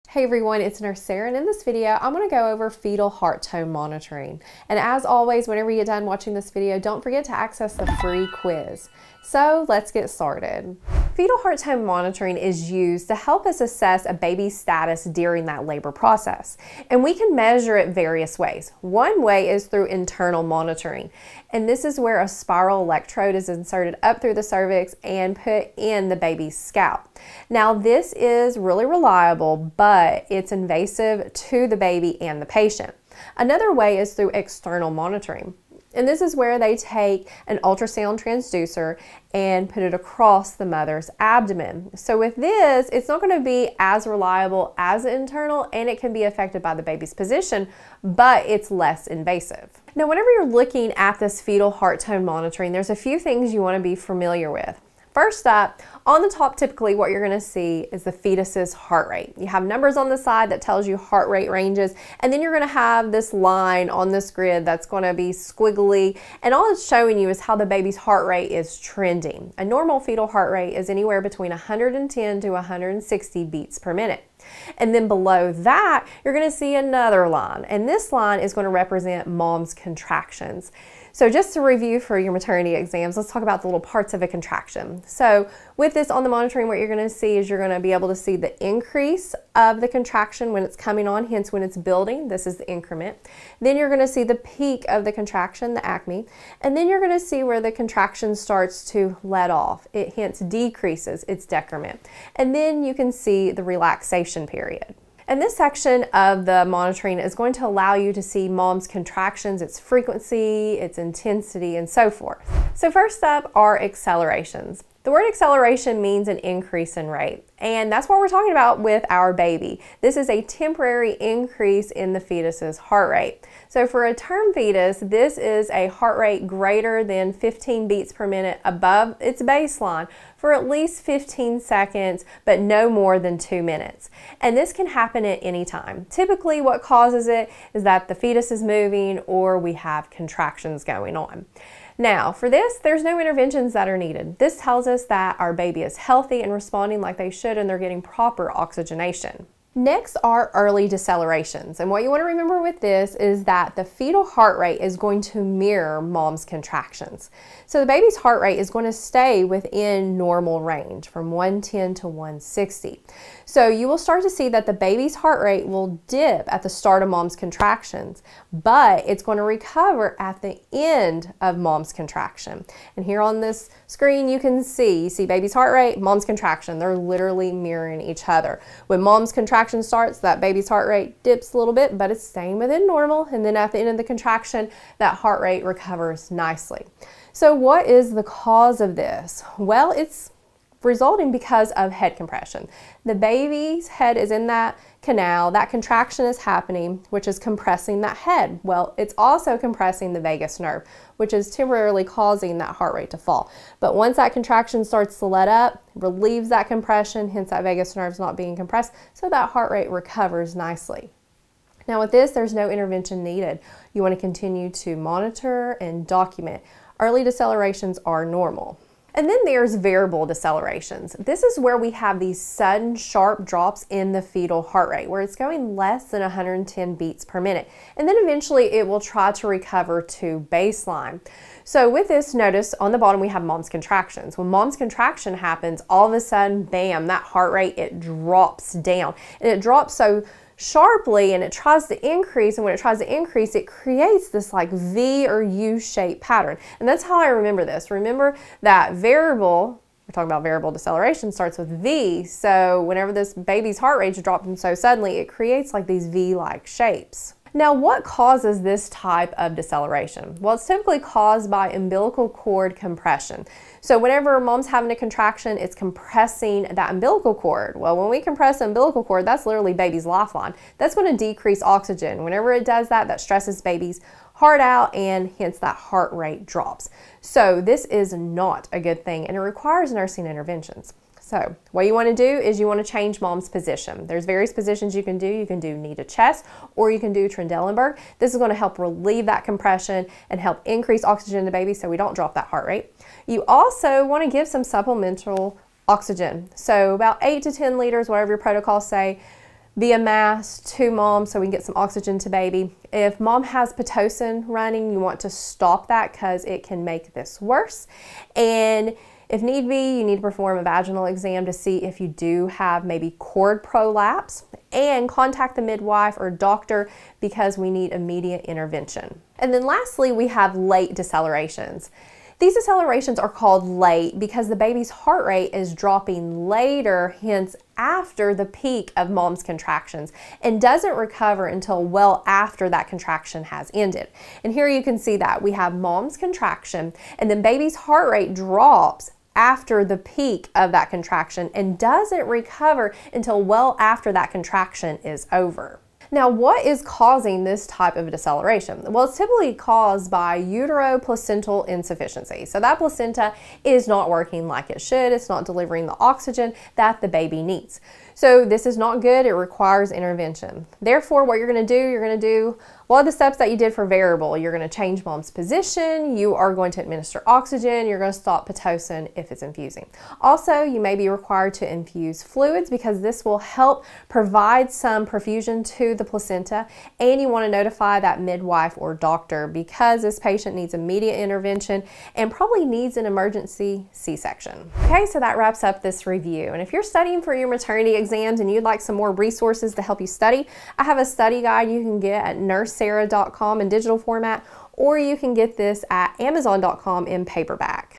Fetal Heart Rate Monitoring Nursing NCLEX: Accelerations, Decelerations - Early, Variable, Late
Fetal heart tone monitoring nursing NCLEX review for nursing school students, nurses, and more!

Learn some of the core concepts you need to remember with fetal heart rate monitoring.

In this video, you'll learn about fetal heart rate accelerations, as well as early decelerations, variable decelerations, and late decelerations, along with the causes and potential nursing interventions for each one.

Early decelerations mirror mom's contractions. Variable decelerations create a "v" shape on the baby's heart monitor. Late decelerations occur AFTER mom's uterine contraction peak.